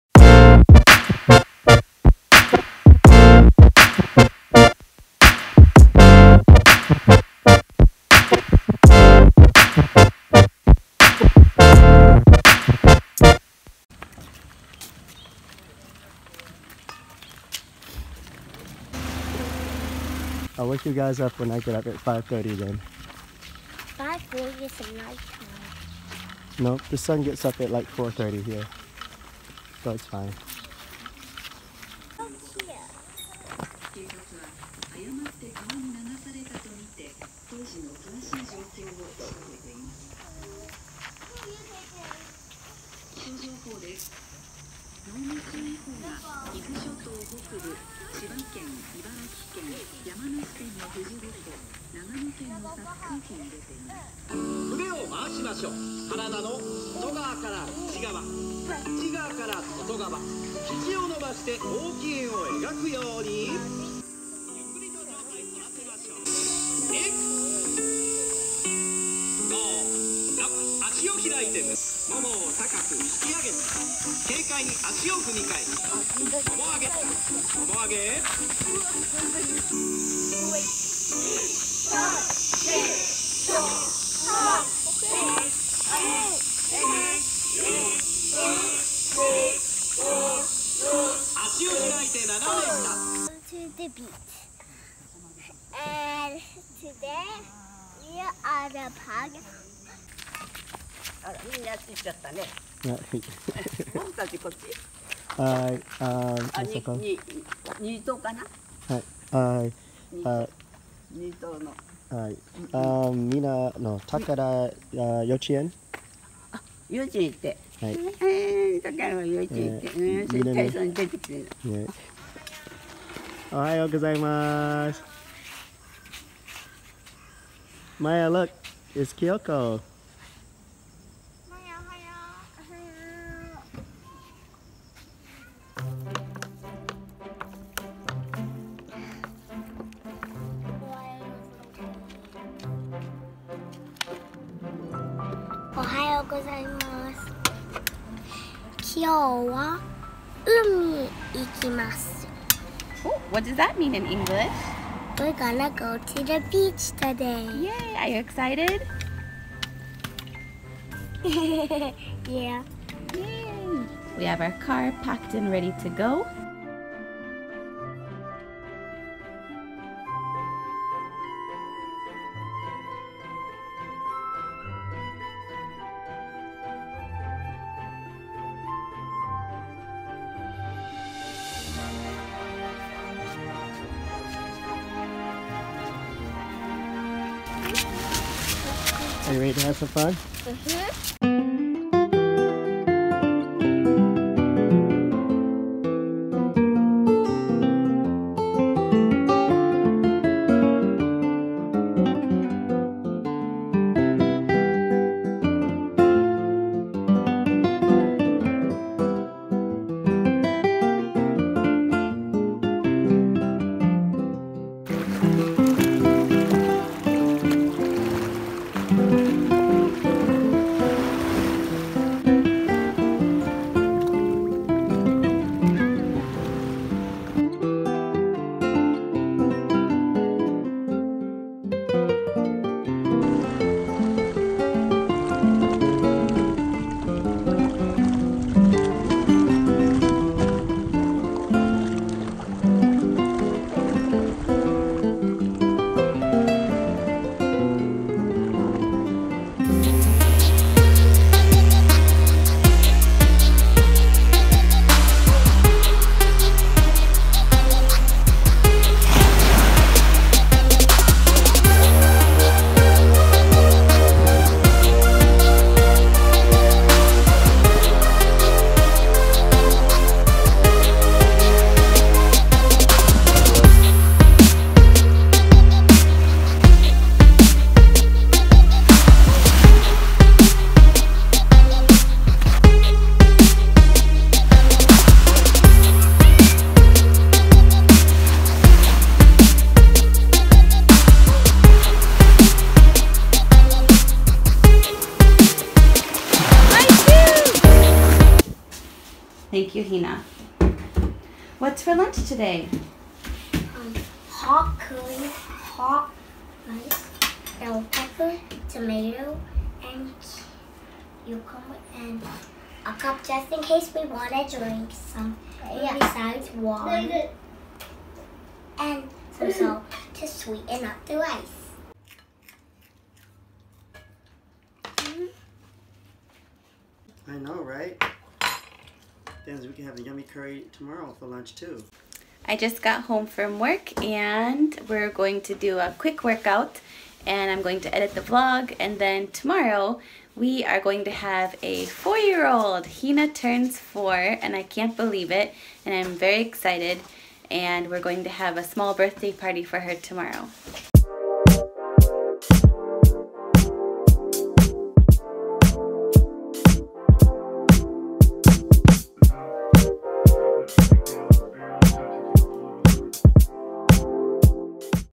Visiting a Japanese Beach
Today we took the girls to the beach for probably the last visit of the summer.